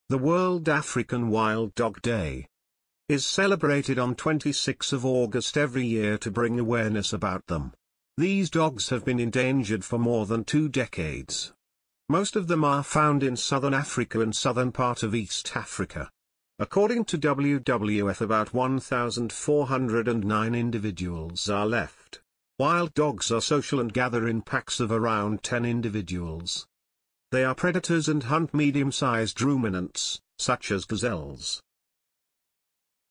🐶World African Wild Dog Day I African wild dog I wild animals I Wild dog
The wild dog is one of the world’s most endangered mammals. The largest populations remain in southern Africa and the southern part of East Africa (especially Tanzania and northern Mozambique).